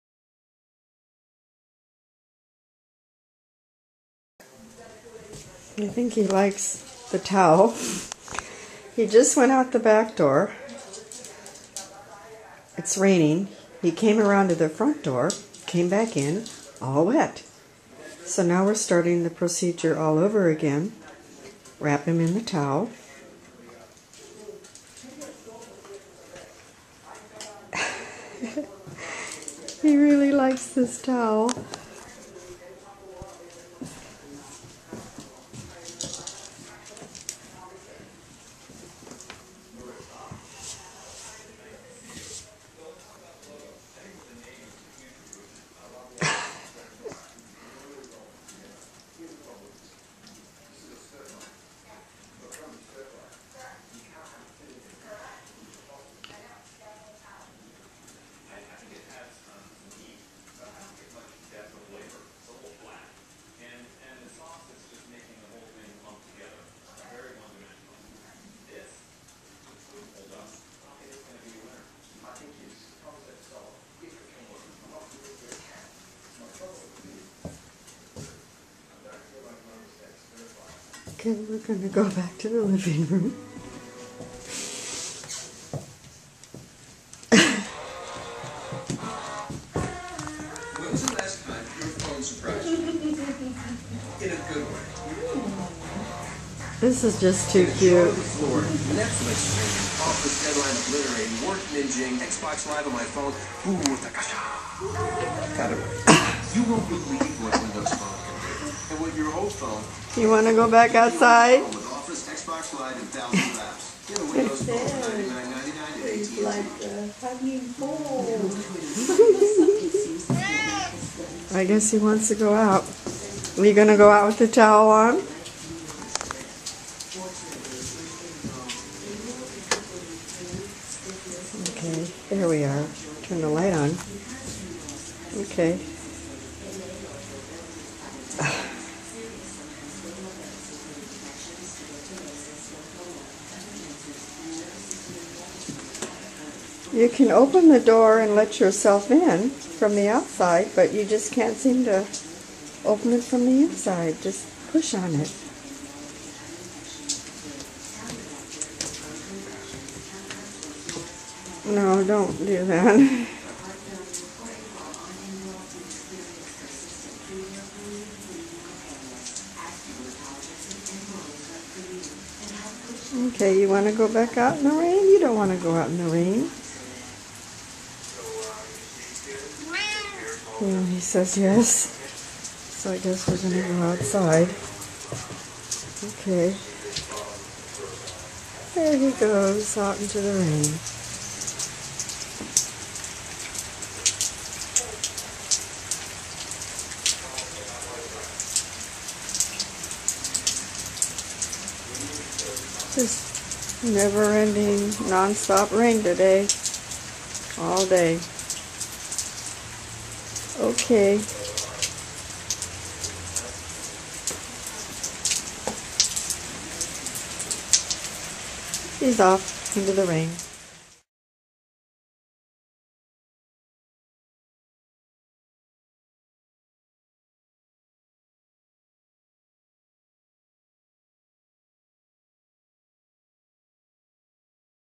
Sam the Cat on a Very Rainy Night - Part 3
Here is the next segment of Sam on a very rainy night in March (Sunday, 3-20-11).  He hates rain but keeps going outside to be in it, then comes back inside soaking wet.  I think he likes being wrapped in the towel and is maybe playing a game with me!  :-)